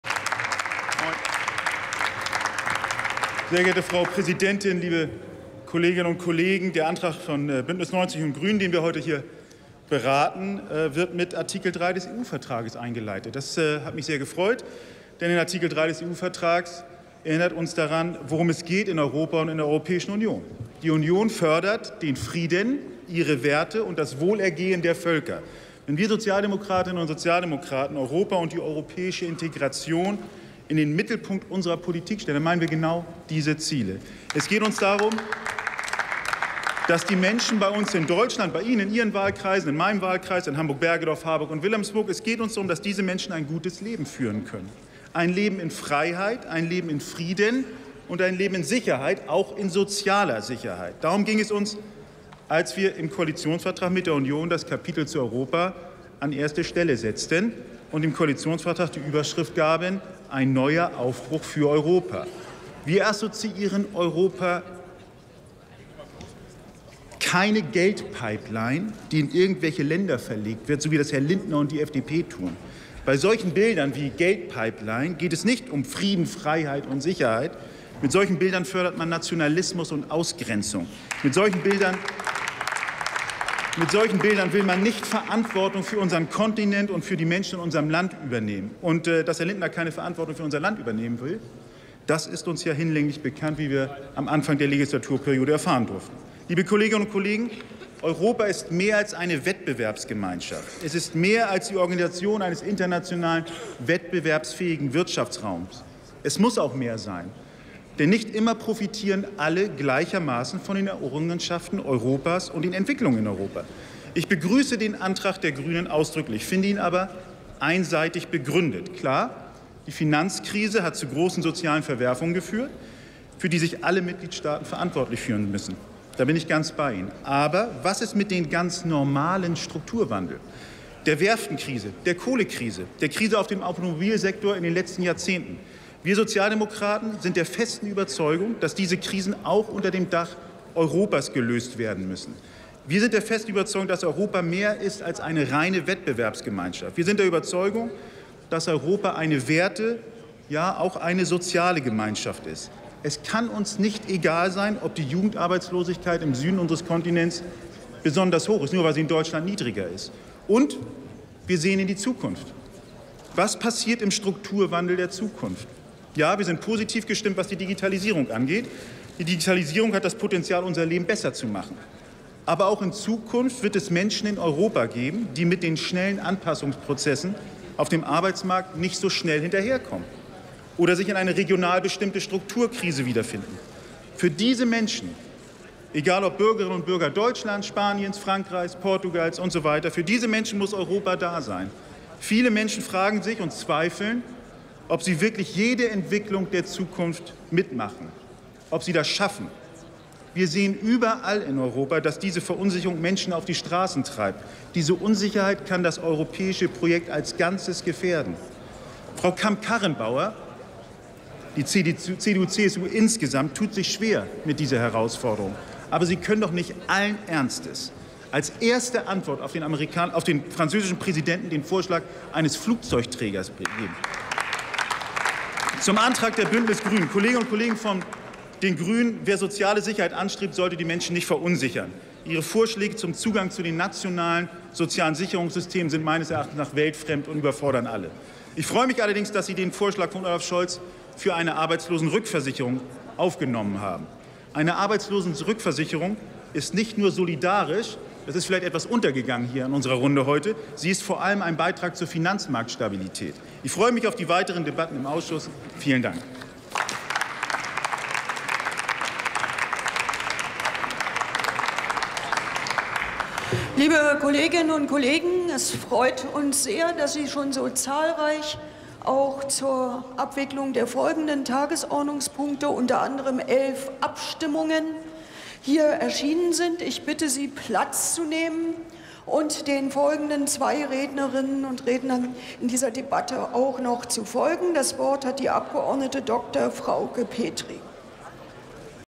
Metin Hakverdi: Soziale Absicherung europaweit [Bundestag 04.04.2019]
Für ein Europa das schützt – Soziale Absicherung europaweit garantieren
Drucksache 19/8287

19. Deutscher Bundestag
92. Plenarsitzung
Tagesordnungspunkt: Soziale Absicherung europaweit
Redner: Hakverdi, Metin, SPD
Partei: Sozialdemokratische Partei Deutschlands (SPD)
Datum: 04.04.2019
Plenarprotokoll:
UVID:7341635